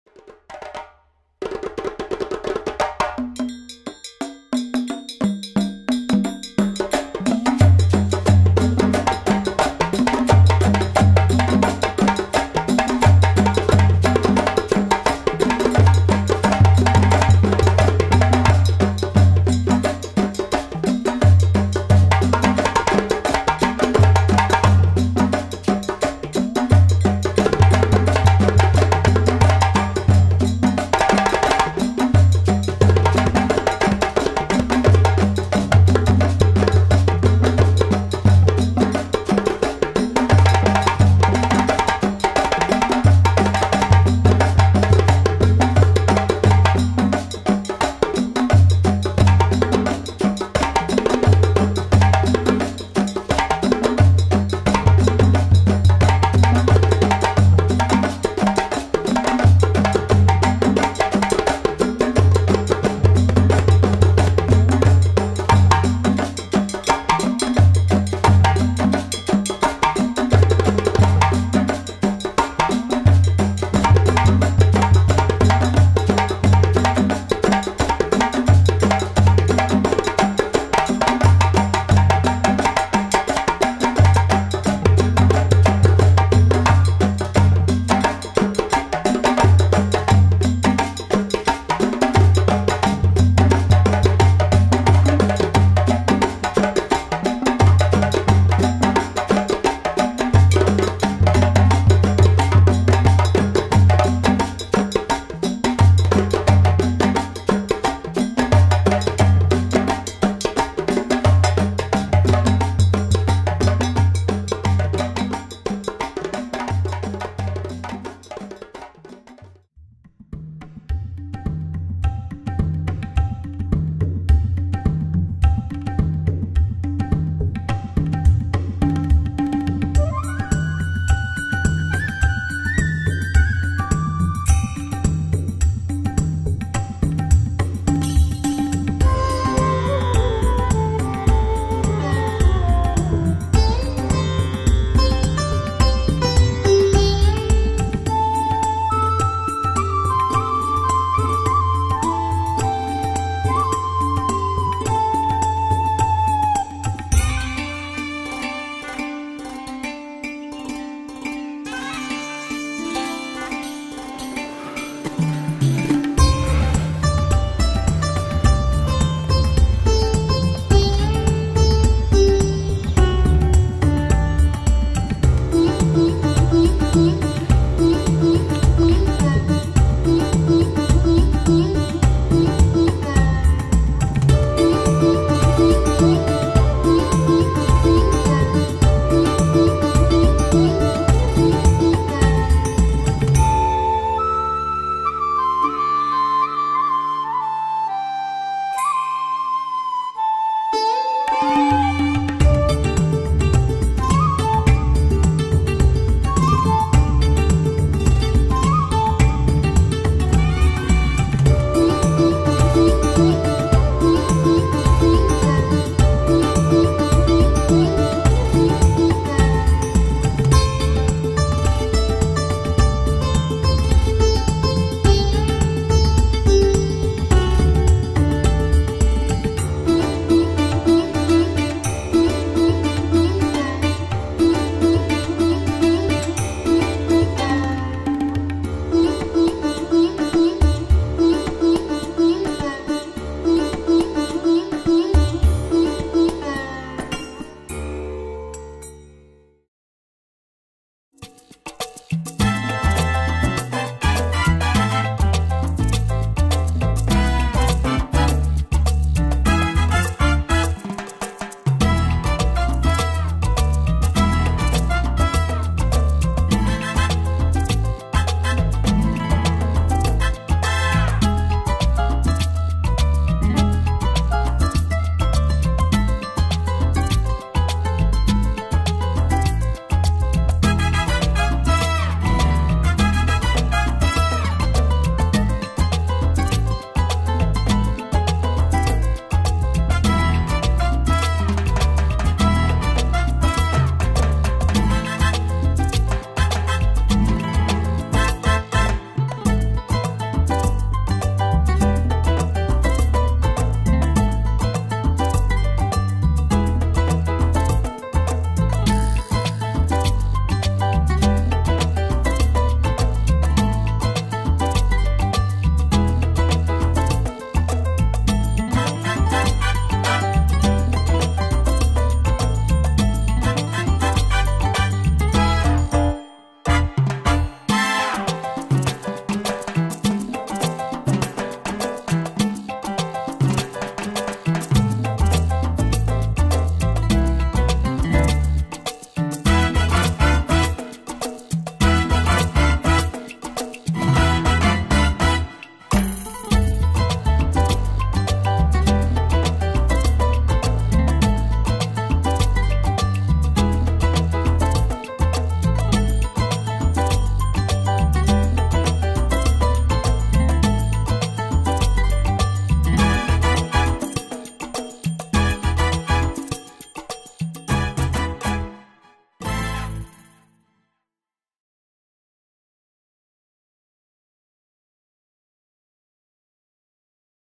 Animals Of The Wild: A Collection Of The Various Animals Living In The Wild HD
A short educational video for kids  depicting some of the animals ( with names) that inhabit the wild